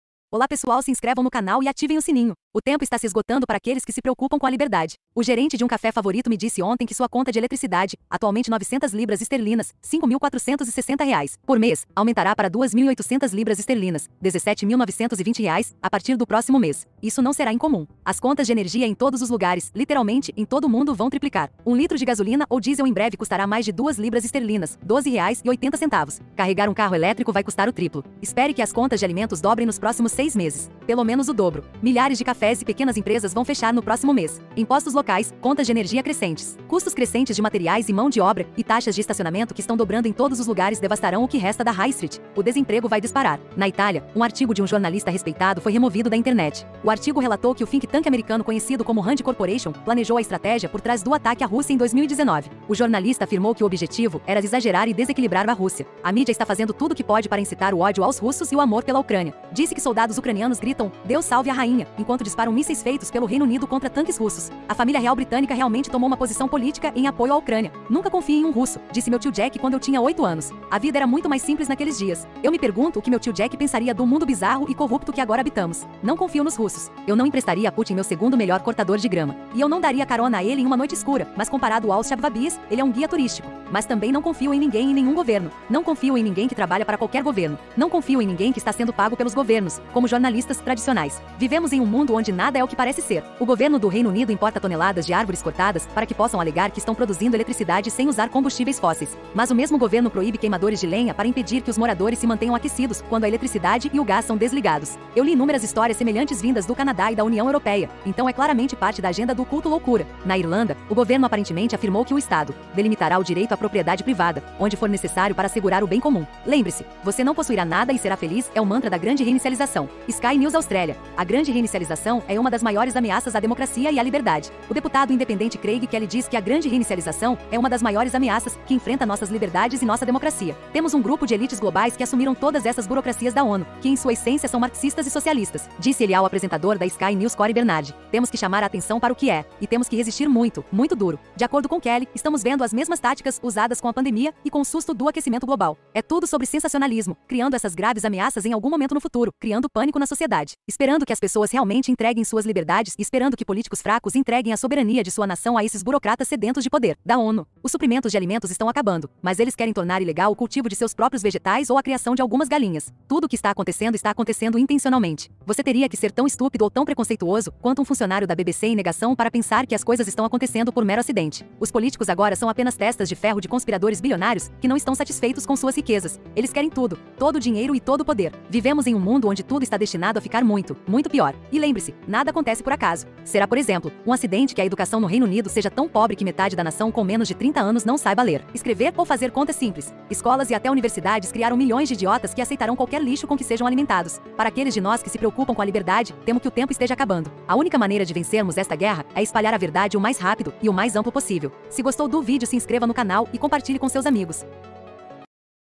O Que Vai Acontecer Ate 2023?Escravidão Ate 2023(Reflexão)
O gerente de um café favorito me disse ontem que sua conta de eletricidade, atualmente £ 900 [R$ 5.460,00] por mês, aumentará para £ 2.800 [R$ 17.920,00] a partir do próximo mês. Isso não será incomum. As contas de energia em todos os lugares [literalmente, em todo o mundo] vão triplicar. Um litro de gasolina ou diesel em breve custará mais de £ 2 [R$ 12,80]. Carregar um carro elétrico vai custar o triplo. Espere que as contas de alimentos dobrem nos próximos seis meses. Pelo menos o dobro.
Milhares de cafés e pequenas empresas vão fechar no próximo mês. Impostos locais, contas de energia crescentes, custos crescentes de materiais e mão de obra e taxas de estacionamento que estão dobrando em todos os lugares devastarão o que resta da High Street. O desemprego vai disparar. (…)

Na Itália, um artigo de um jornalista respeitado foi removido da internet. O artigo relatou que o think tank americano conhecido como Rand Corporation planejou a estratégia por trás do ataque à Rússia em 2019. O jornalista afirmou que o objetivo era ‘exagerar e desequilibrar’ a Rússia.

A mídia está fazendo tudo o que pode para incitar o ódio aos russos e o amor pela Ucrânia. Diz-se que soldados ucranianos gritam “Deus salve a rainha” enquanto disparam mísseis feitos pelo Reino Unido contra tanques russos. A família real britânica realmente tomou uma posição política em apoio à Ucrânia…

“Nunca confie em um russo”, disse meu tio Jack quando eu tinha oito anos. A vida era muito mais simples naqueles dias. Eu me pergunto o que meu tio Jack pensaria do mundo bizarro e corrupto que agora habitamos.

Não confio nos russos. Eu não emprestaria a Putin meu segundo melhor cortador de grama. E eu não daria carona a ele em uma noite escura, mas comparado aos Schwabbies, ele é um guia turístico.

Mas também não confio em ninguém em nenhum governo. Não confio em ninguém que trabalha para qualquer governo. Não confio em ninguém que está sendo pago pelos governos (como jornalistas “tradicionais”).

Vivemos em um mundo onde nada é o que parece ser. O governo do Reino Unido importa toneladas de árvores cortadas para que possam alegar que estão produzindo eletricidade sem usar combustíveis fósseis. Mas o mesmo governo proíbe queimadores de lenha para impedir que os moradores se mantenham aquecidos quando a eletricidade e o gás são desligados.

[Eu li inúmeras histórias semelhantes vindas do Canadá e da União Europeia, então é claramente parte da agenda do Culto] Loucura!

Na Irlanda, o Governo aparentemente afirmou que ‘o Estado… delimitará o direito à propriedade privada, onde for necessário para assegurar o bem comum’.
O deputado independente Craig Kelly diz que a Grande Reinicialização é “uma das maiores ameaças” que enfrenta nossas liberdades e nossa democracia.

“Temos um grupo de elites globais que assumiram todas essas burocracias da ONU, que em sua essência são marxistas e socialistas”, disse ele ao apresentador da Sky News Cory Bernardi.

“Temos que chamar a atenção para o que é, e temos que resistir muito, muito duro.”

De acordo com Kelly, estamos vendo as “mesmas táticas” usadas com a pandemia e com o “susto do aquecimento global”.

“É tudo sobre sensacionalismo, criando essas graves ameaças em algum momento no futuro, criando pânico na sociedade, esperando que as pessoas realmente entreguem suas liberdades e esperando que políticos fracos entreguem a soberania de sua nação a esses burocratas sedentos de poder da ONU.”

Os suprimentos de alimentos estão acabando, mas eles querem tornar ilegal o cultivo de seus próprios vegetais ou a criação de algumas galinhas. Tudo o que está acontecendo está acontecendo intencionalmente.

Você teria que ser tão estúpido ou tão preconceituoso quanto um funcionário da BBC em negação para pensar que as coisas estão acontecendo por mero acidente.

Os políticos agora são apenas testas de ferro de conspiradores bilionários que não estão satisfeitos com suas riquezas. Eles querem tudo: todo o dinheiro e todo o poder.

Vivemos em um mundo onde tudo está destinado a ficar muito, muito pior. E lembre-se: nada acontece por acaso.

Será, por exemplo, um acidente que a educação no Reino Unido seja tão pobre que metade da nação com menos de 30 anos não saiba ler, escrever ou fazer contas simples?

Escolas e até universidades criaram milhões de idiotas que aceitarão qualquer lixo com que sejam alimentados.

Para aqueles de nós que se preocupam com a liberdade, temo que o tempo esteja acabando.

A única maneira de vencermos esta guerra é espalhar a verdade o mais rápido e o mais amplo possível.